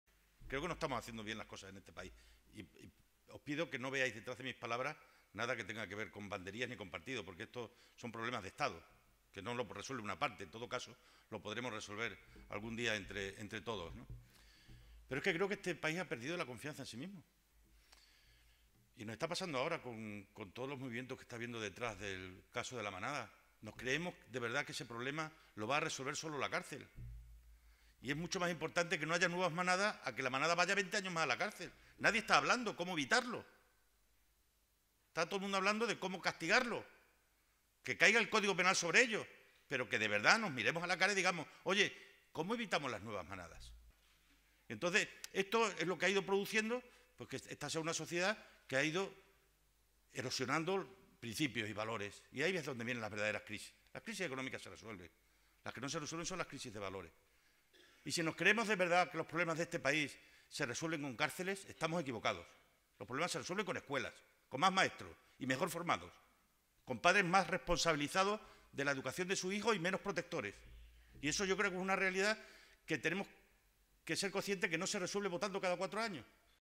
Vara: Es "mucho más importante que no haya nuevas manadas"
El presidente de la Junta, Guillermo Fernández Vara, se ha referido al caso de La Manada y ha dicho que este "problema no lo va a resolver solo la cárcel", al tiempo que ha reconocido que es "mucho más importante que no haya nuevas manadas". "Nadie está hablando de cómo evitarlo. Está todo el mundo hablando de cómo castigarlos", ha apuntado.